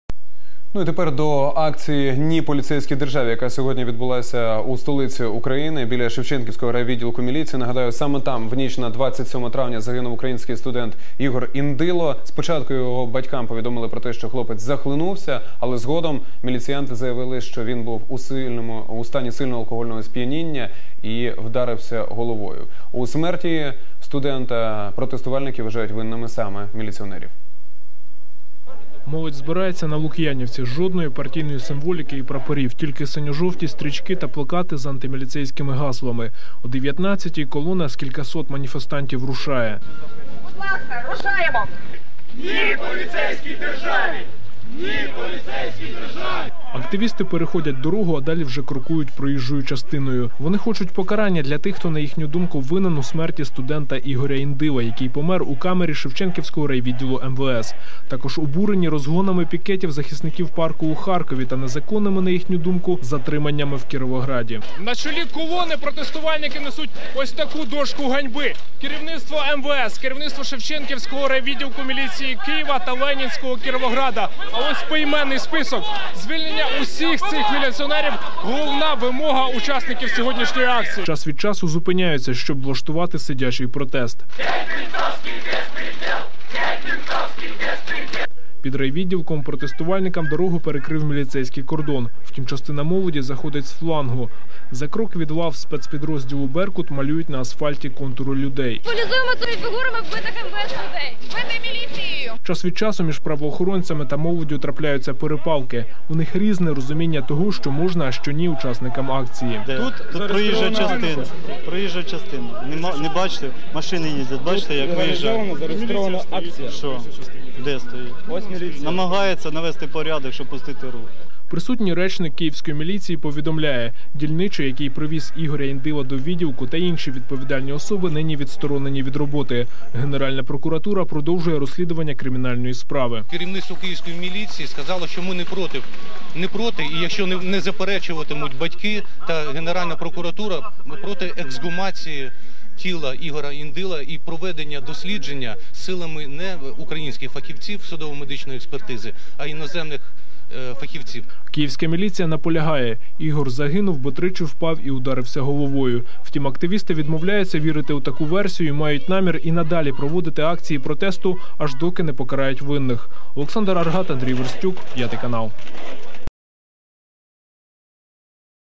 Акція протесту Ні поліцейській державі 10/06/2010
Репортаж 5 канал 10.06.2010

10 червня відбулася акція «Ні поліцейській державі». Учасники акції пішою ходою пройшли від м. Лук'янівська до Шевченківського райвідділу міліції (вул. Герцена, 9), де було вбито студента Ігоря Індило. В рамках акції відбувся сидячий протест. Учасники зображували на асфальті контури людей, які стали жертвами міліцейського свавілля.